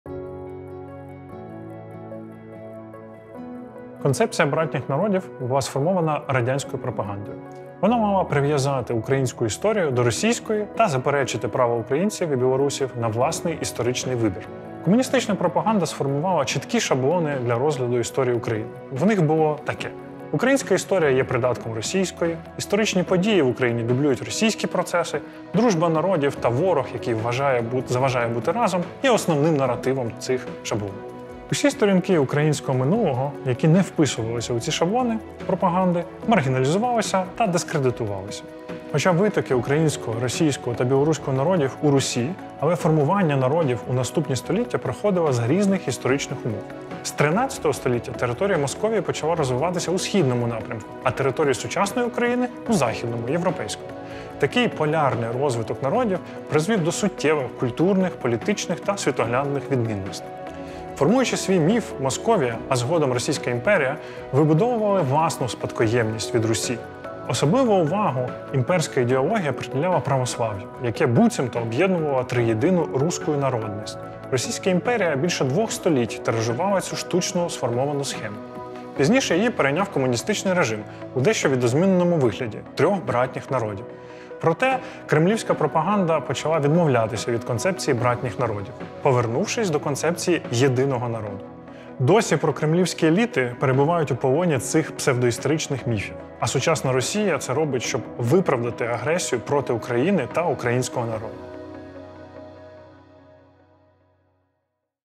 Звідки з'явився міф про "братні народи"?
Продовжуємо розвінчувати міфи російської пропаганди. Сьогоднішній ролик спростовує міф про «братні народи». Ви дізнаєтесь, коли була створена концепція «братніх народів» та чому формування українського та російського народів проходило за різних історичних умов.

Ролик створено в рамках спільного просвітницького проєкту Медіацентру Україна та Українського інституту національної пам'яті. Він розвінчує міфи російської пропаганди, а також роз'яснює, що таке «рашизм» та які він має ознаки.

Проєкт складається з двох серій відеороликів, які допоможуть дізнатися більше про історію протистояння рф і України, зруйнують багаторічні міфи про Україну та розкриють природу російських злочинів.